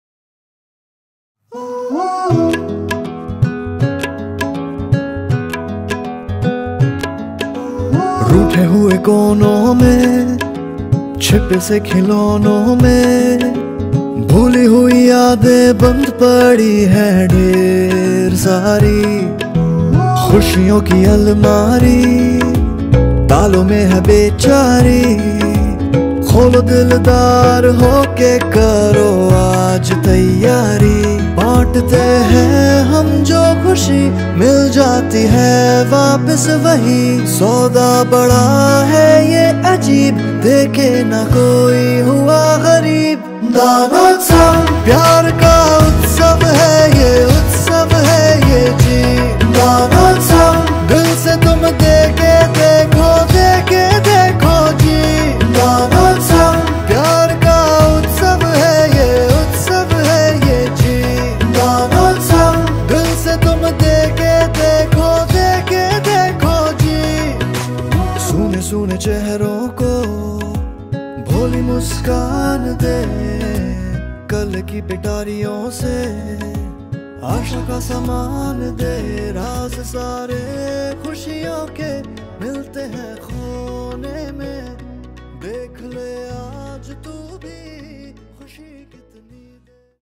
DAAN UTSAV celebration by Treasures of Innocence
Treasures of Innocence celebrated DAAN UTSAV with a road rally including children, women ,youth and school students. They walked together in a vibrant celebration of unity and hope.
Bondhutter Bandhon: A heartfelt exchange of love and care through the sacred thread to the loved ones.
Cultural Programme: Students showcase their talents dance, song, and recitation ,filling in air with joy.